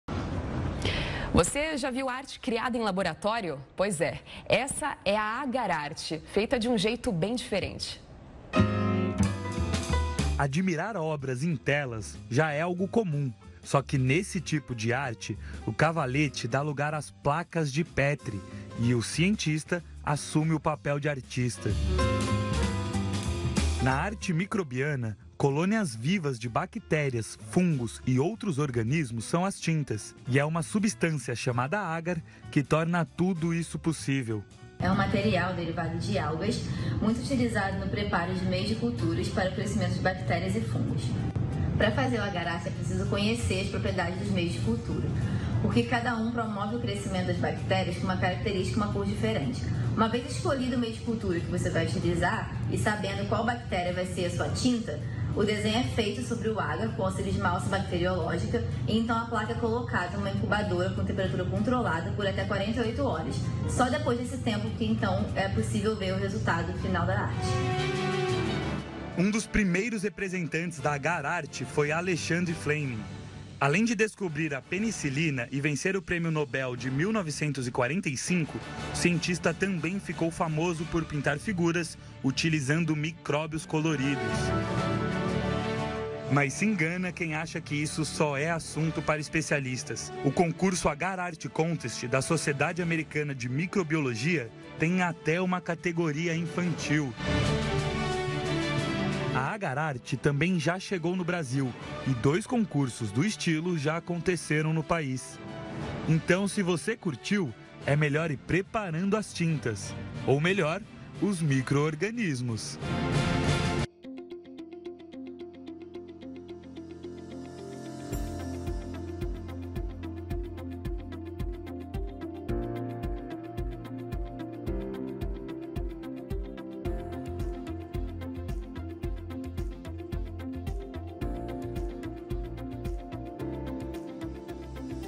matéria da Record News sobre Agar Art
Confira a matéria da Record News sobre Agar Art, que contou com a participação da Analista de Qualidade Sênior, Andreia Damasco.